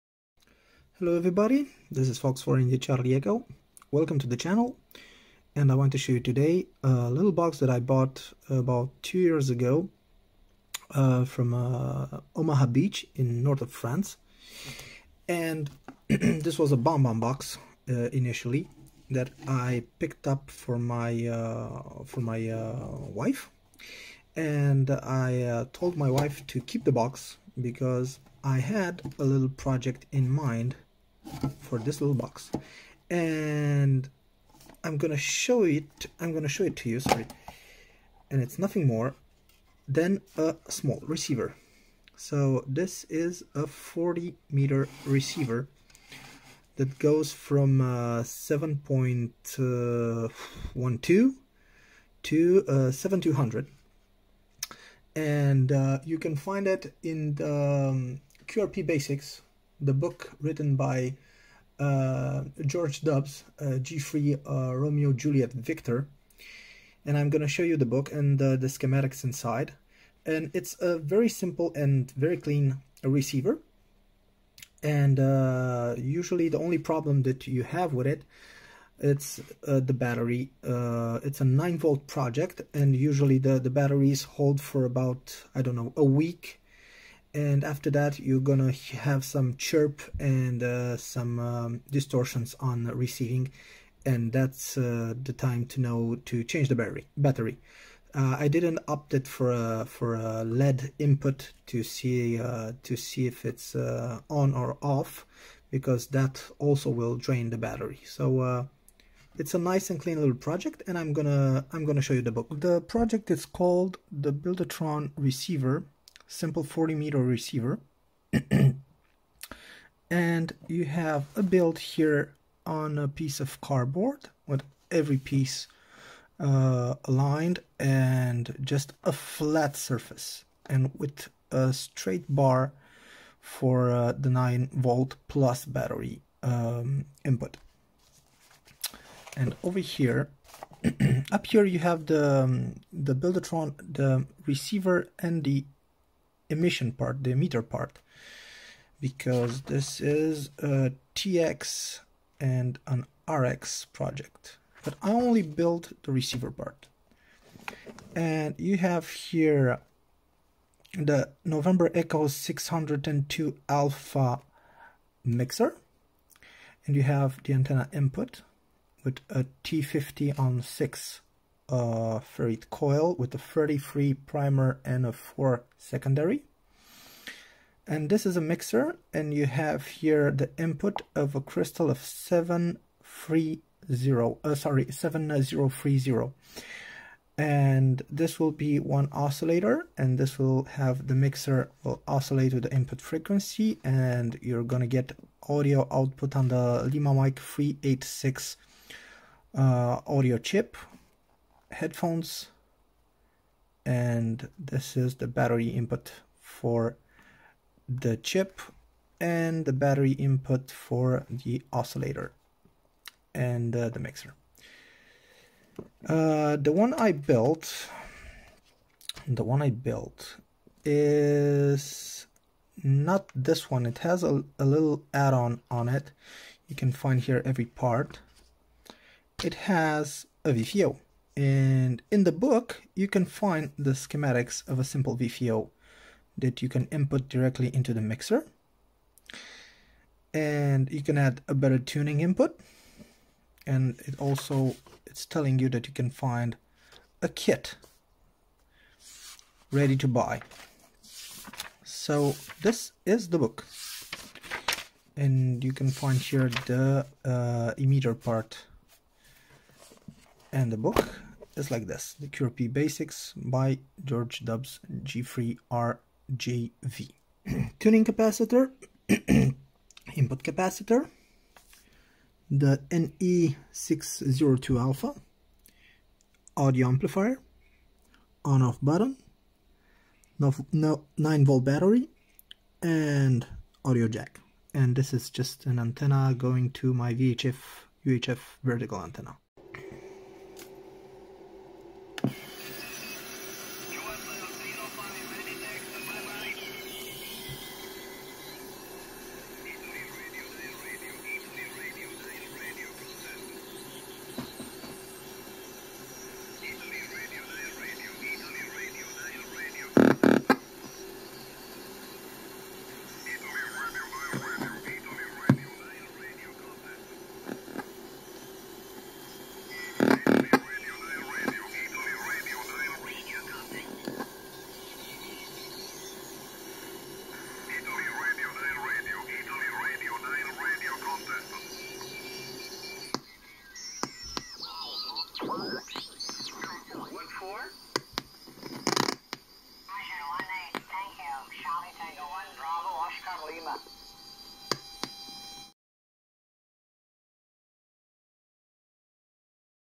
40m cw/ssb receiver
A simple 7mHz receiver build with a NE602a mixer and a LM386 audio amplifier.
The schematics are available in "qrp basics" by George Dobbs G3RJV